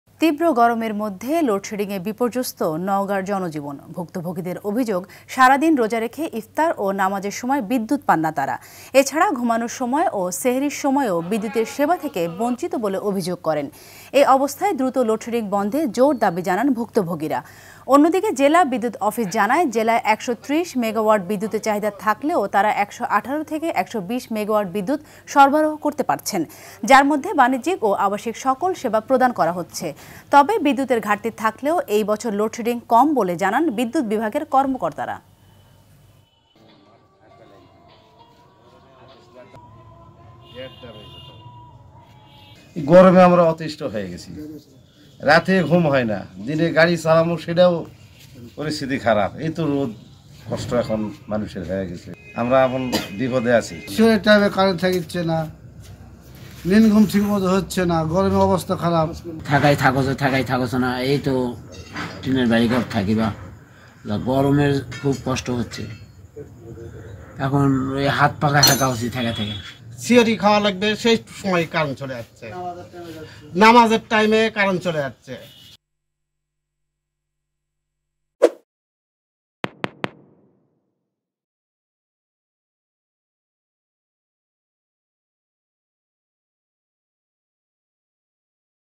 সারা দিন রোজা রেখে ইফতার ও নামাযের সময় বিদ্যুৎ পাননা নওগাঁবাসী | Load Shedding | Rtv News
সারা দিন রোজা রেখে ইফতার ও নামাযের সময় বিদ্যুৎ পাননা নওগাঁবাসী